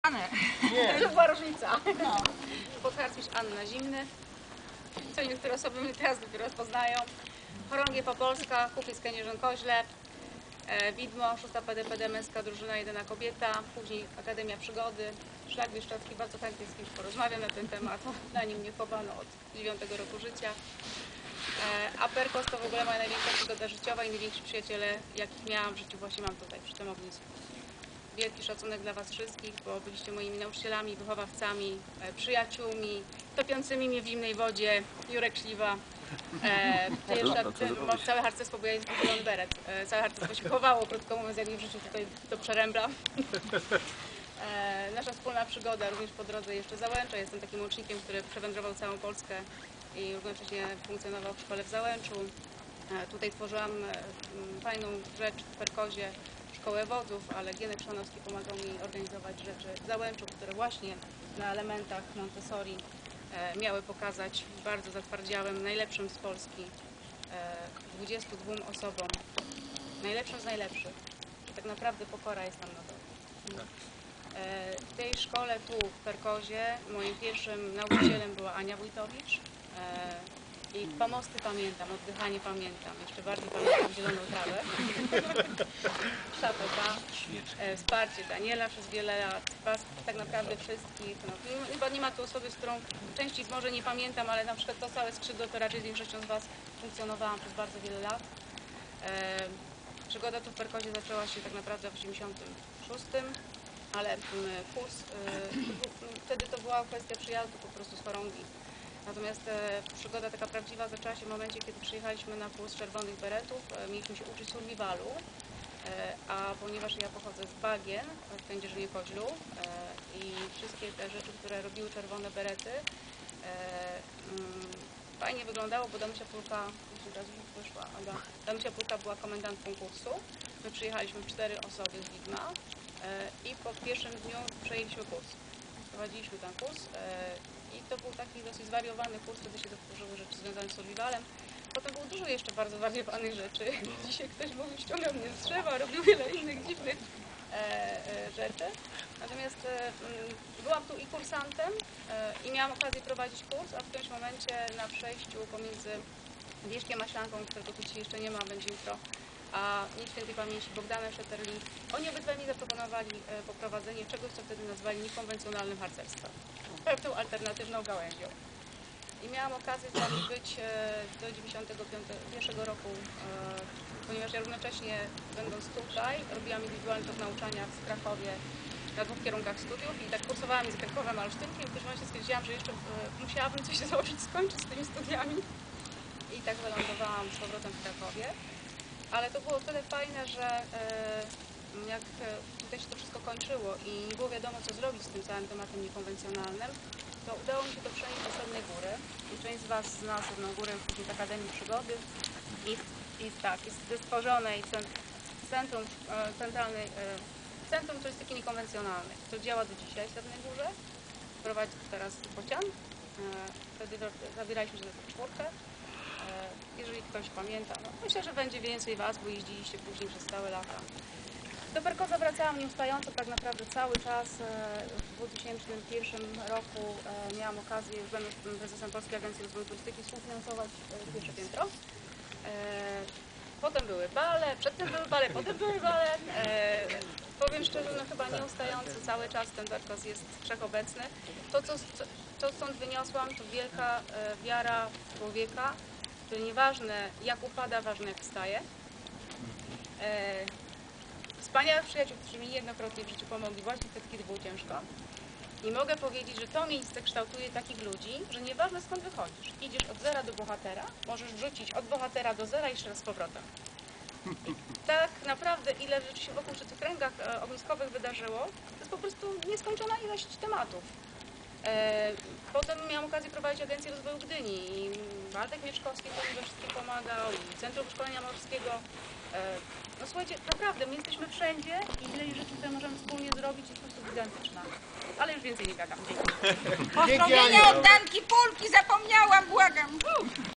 Ognisko XXX Lecia SHS Perkoz 4/8
Ognisko otworzyło spotkanie instruktorów Szkoły Harcerstwa Starszego Perkoz, które odbyło się w dniach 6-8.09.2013 roku